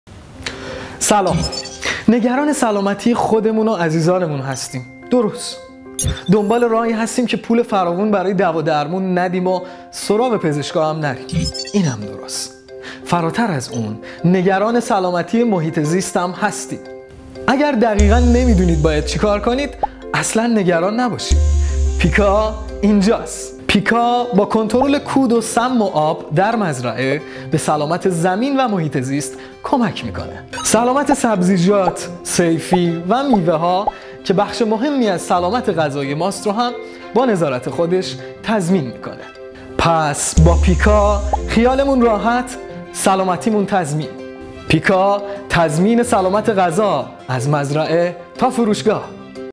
Teaser of Pika Company (organic products)
تیزر تبلیغاتی شرکت پیکا
شرکت پیکا تولید محصول ارگانیک
ساخت تیزر تبلیغاتی و موشن گرافیک